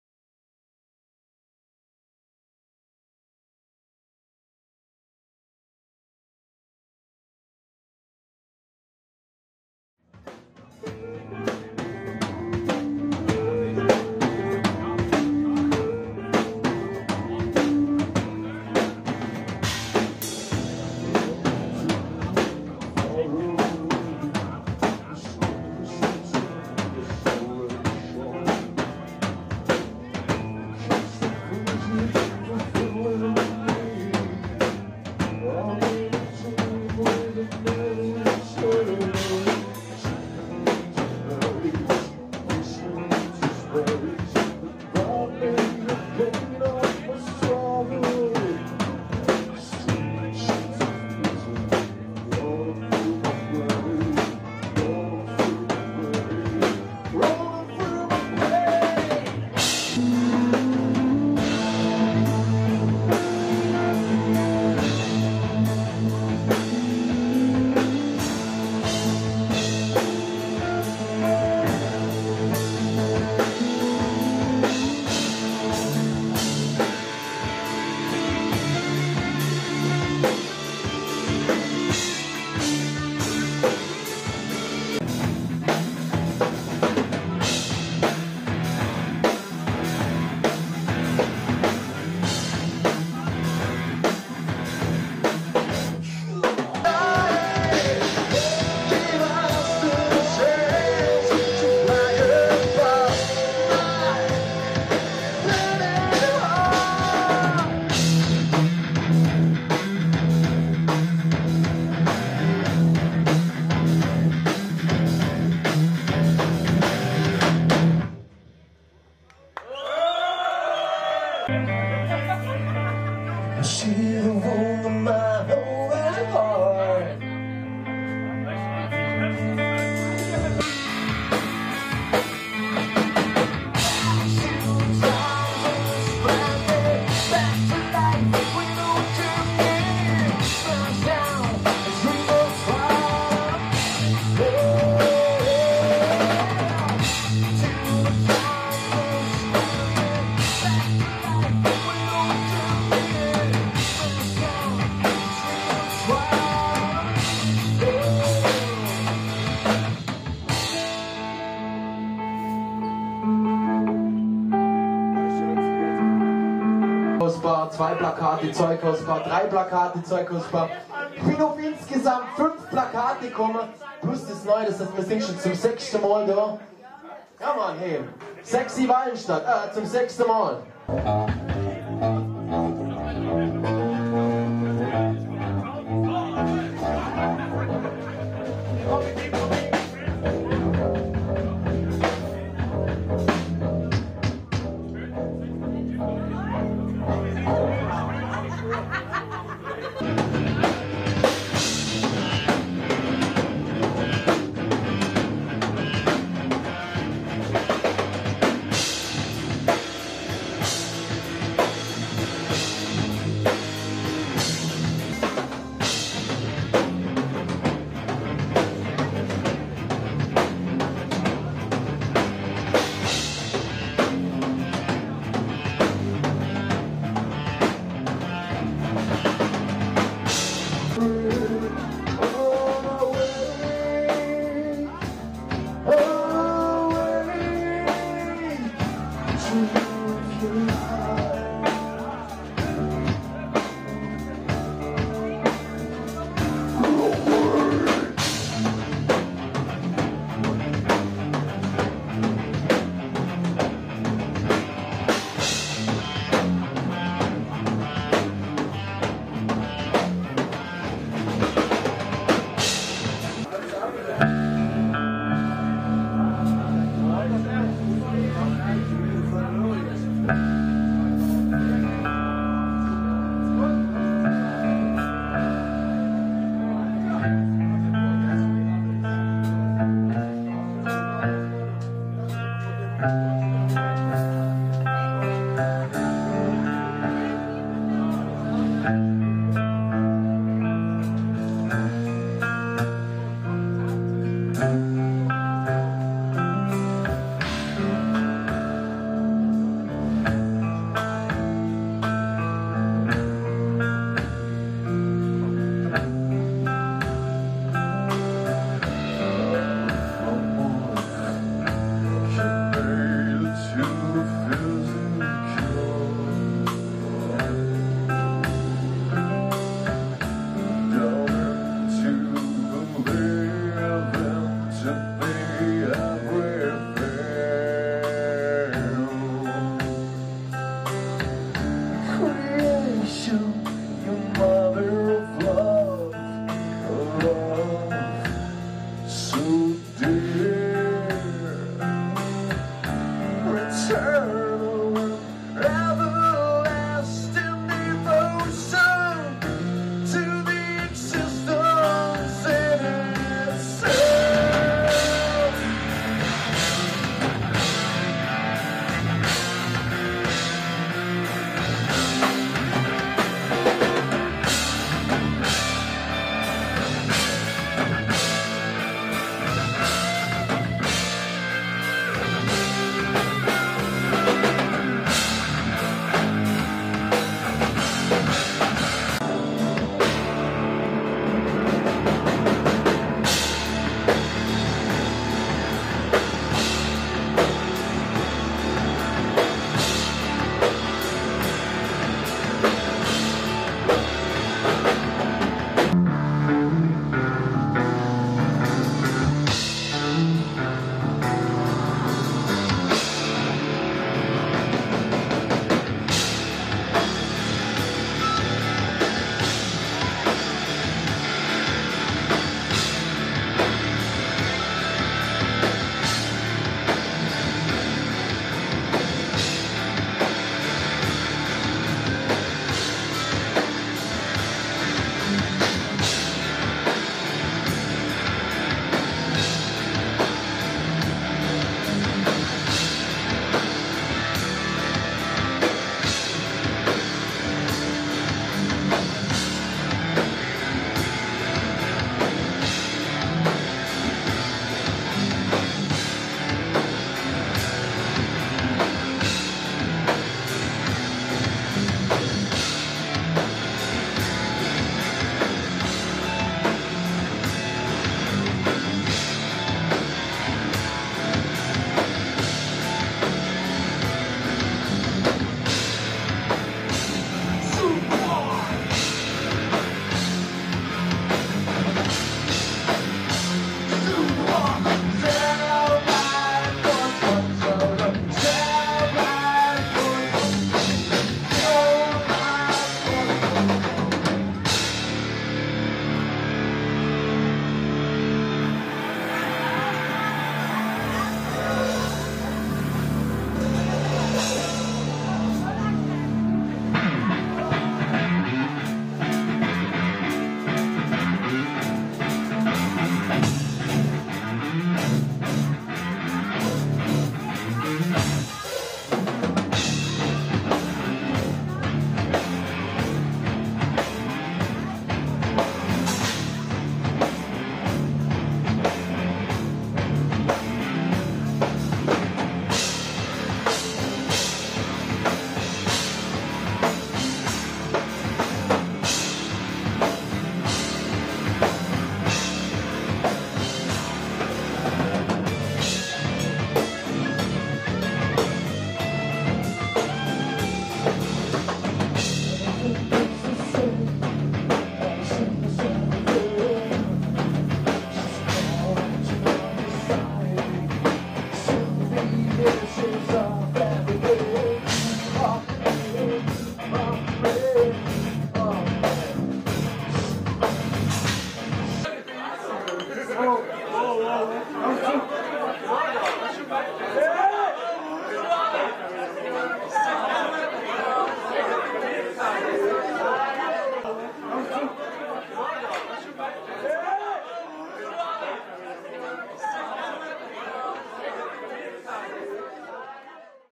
Soul Tonic ät Zeughausbar
Soul Tonic rockin Zeughausbar in Walenstadt.